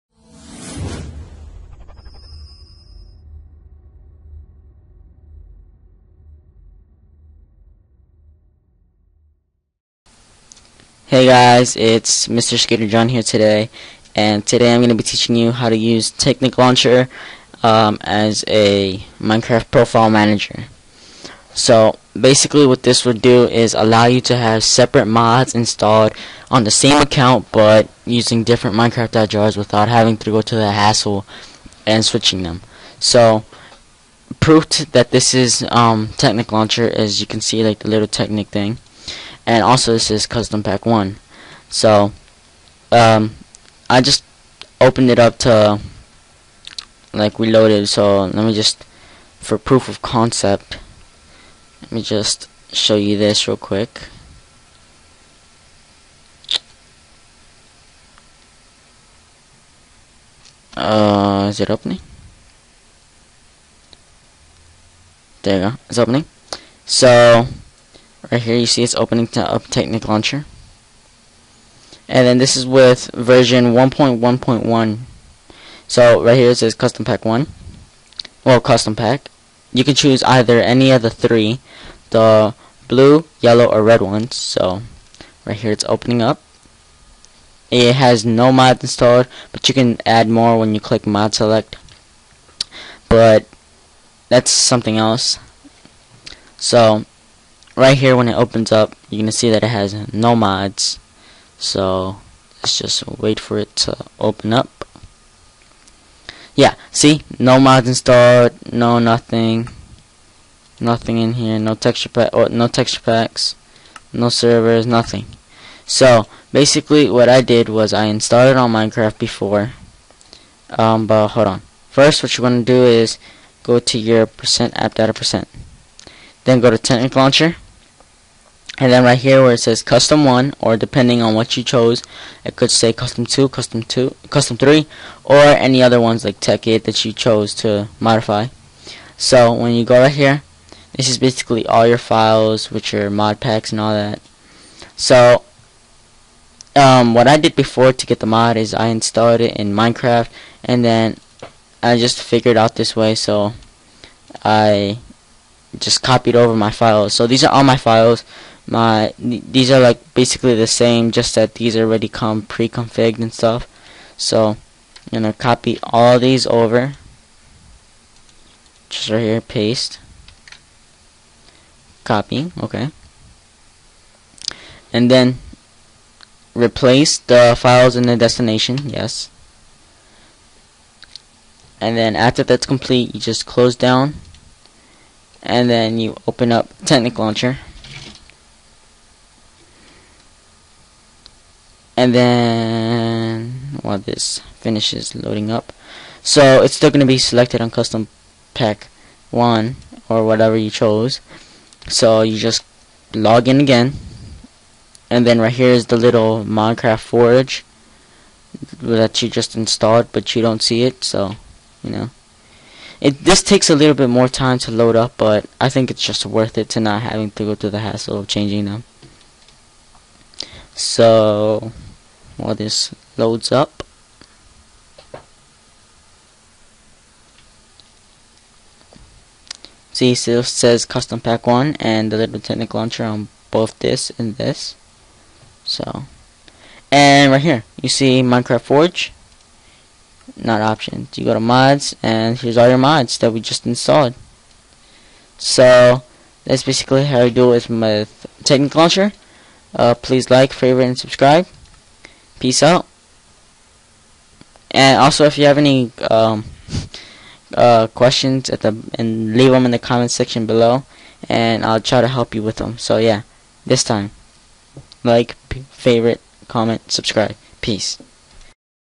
Minecraft Technic Profile Launcher
A Tutorial On How To Use Technic Launcher As A Minecraft Profile Manager.
Previous Video
Mystery Video